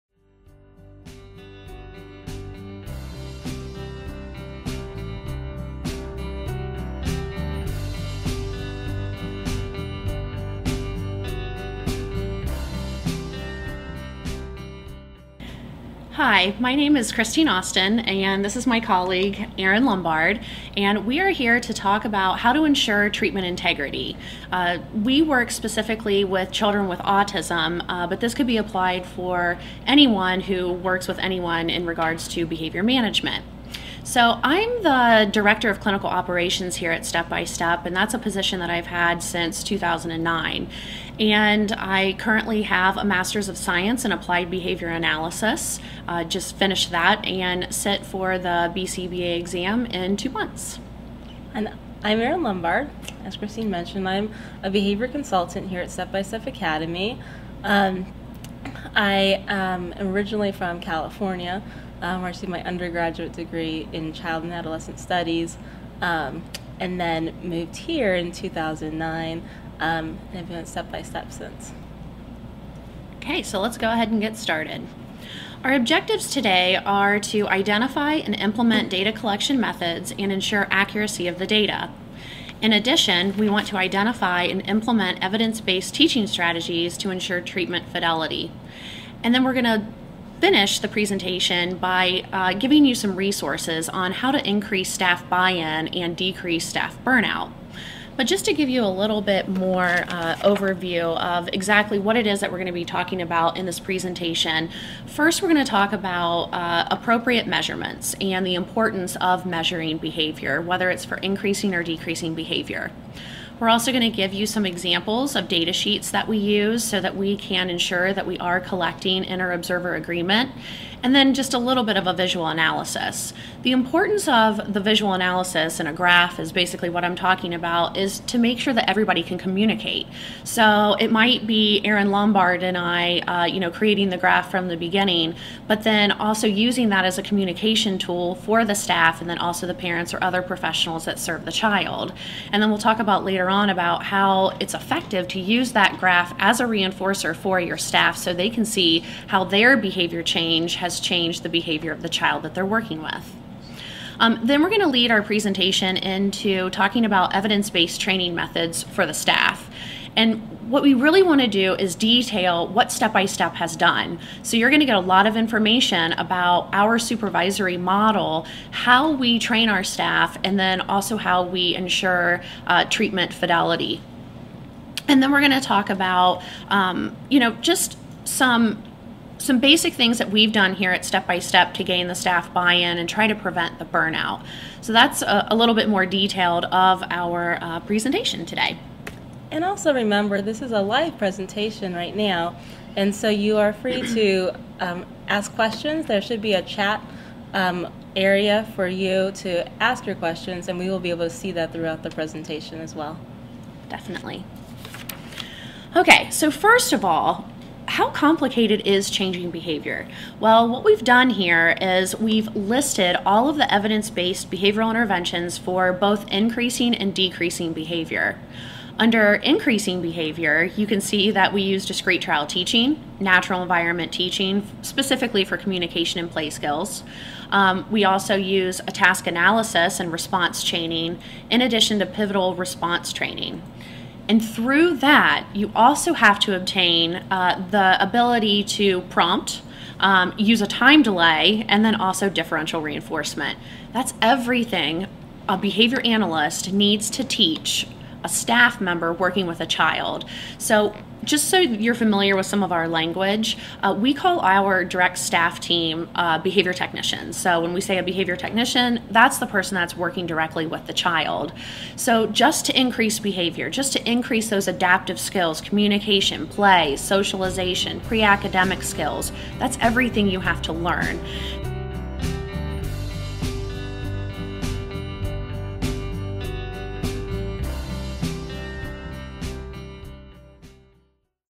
Ensuring Treatment Integrity
This 120-minute recorded webcast provides an overview of the use of applied behavior analytic principles to increase data accuracy and treatment fidelity while teaching techniques to gain staff buy-in. Data collection may be considered the cornerstone of applied behavior analysis. Data determines how behavior analysts make decisions about behavioral interventions. Data is often collected by other staff, not necessarily behavior analysts. Behavior analysts, therefore, must ensure that the collected data is accurate and reliable. Staff must also be trained to implement interventions with fidelity. Common issues in the full implementation of behavior interventions involve inaccurate data recording, observer/"implementation" drift, and inaccurate implementation of behavioral interventions.

Looking for CEUs, Training, or Resources?
We got it!